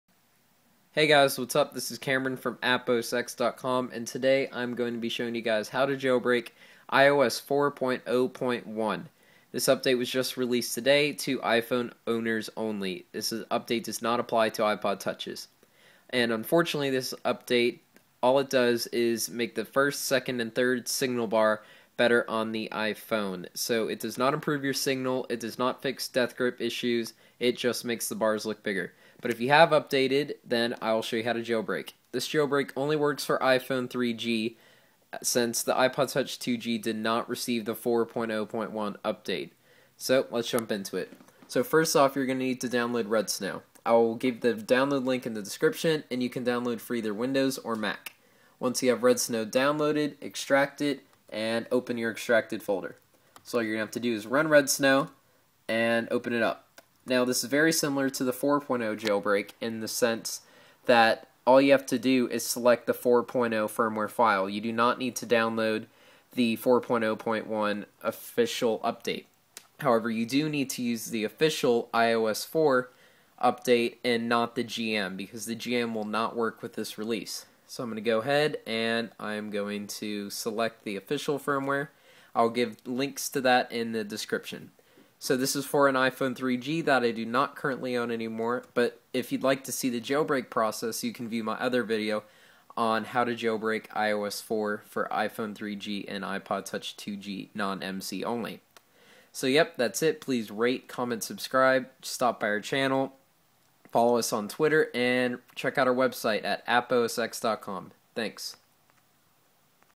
How To: Jailbreak iOS 4.0.1 On Windows and Mac [OFFICIAL] (iPhone 3G only)
Jailbreak iOS 4.0.1 for iPhone 3G.

I know you guys have been waiting such a long time for the new spirit jailbreak, but I can not speed up the release.  We must just wait for comex and the Dev Team.  Sorry :(